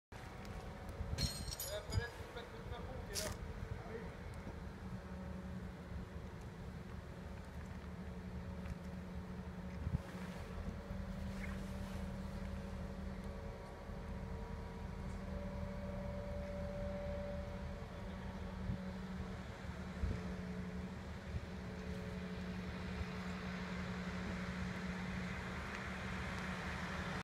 Une grue de 70 m de haut en action à La Chapelle-du-Bois (Sarthe)
Une grue soulève les modules de la future Maison d’assistantes maternelles de La Chapelle-du-Bois dans la Sarthe